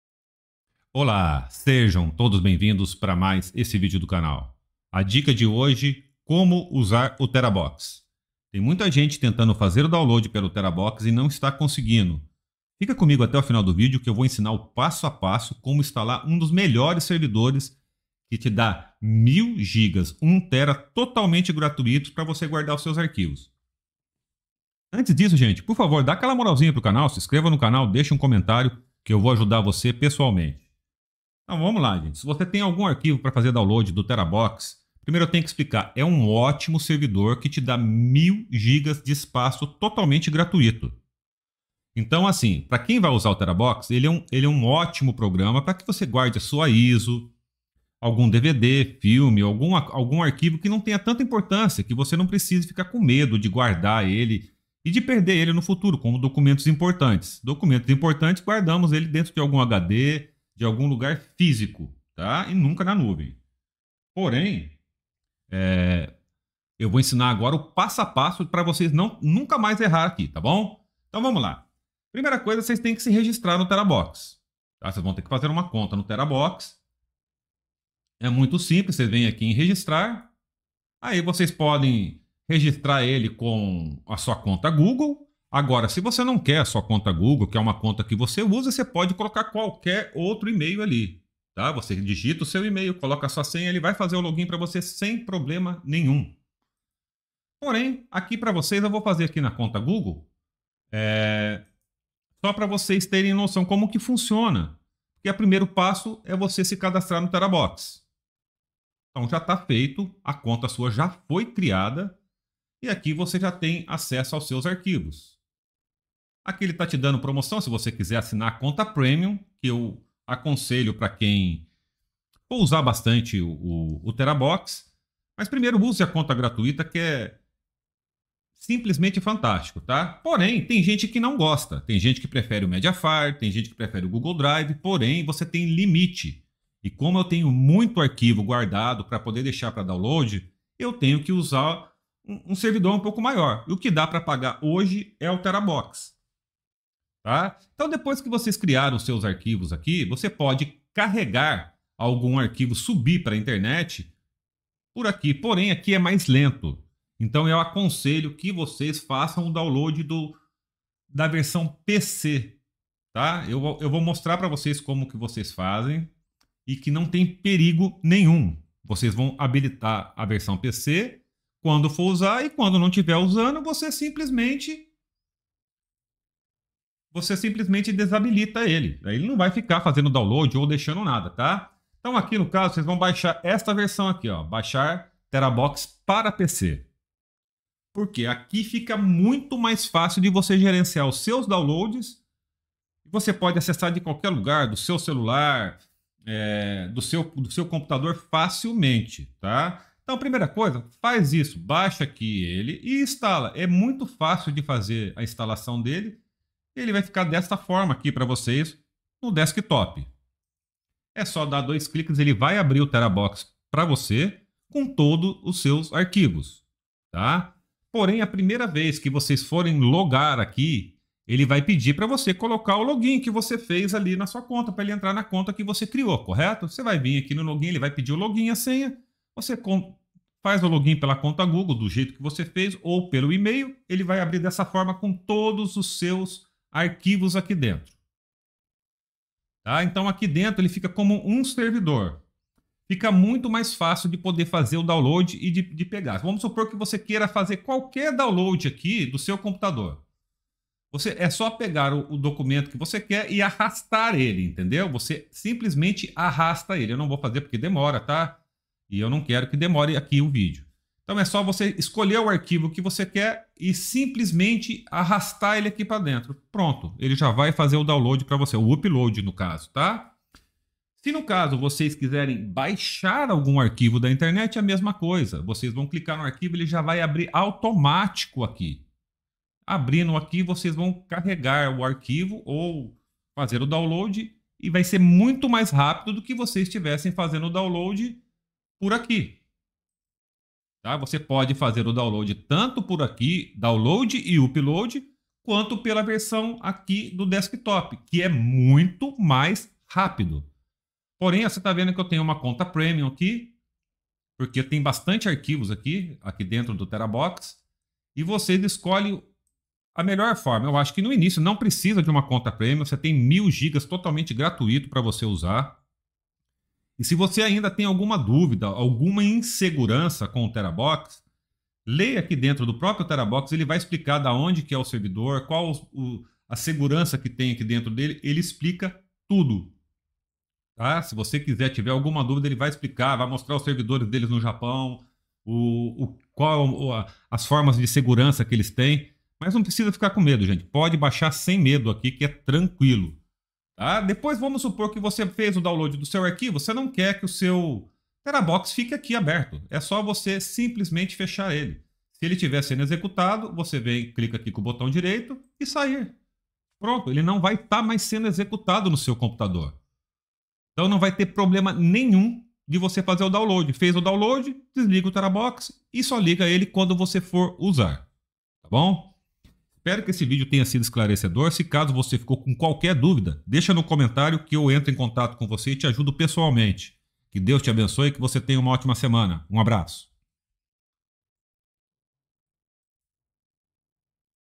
Como usar o Terabox 2024, !000 gigas na nuvem gratis
Faça backup ou compartilhe facilmente seus arquivos na nuvem por meio de nosso aplicativo para desktop, aplicativo móvel ou interface da web. Se você sincronizar ou fazer backup de dados de qualquer dispositivo, todas as alterações serão atualizadas automaticamente em tempo real, para que você possa acessar os arquivos mais recentes a qualquer momento e em qualquer dispositivo.
O TeraBox protege a segurança dos dados
A TeraBox atribui grande importância à segurança dos seus dados e à proteção da sua privacidade, protegendo os seus dados na nuvem através da sua capacidade de armazenamento líder no setor e certificação tecnológica.
👇 Links Importantes 👇
👇 Links Importantes 👇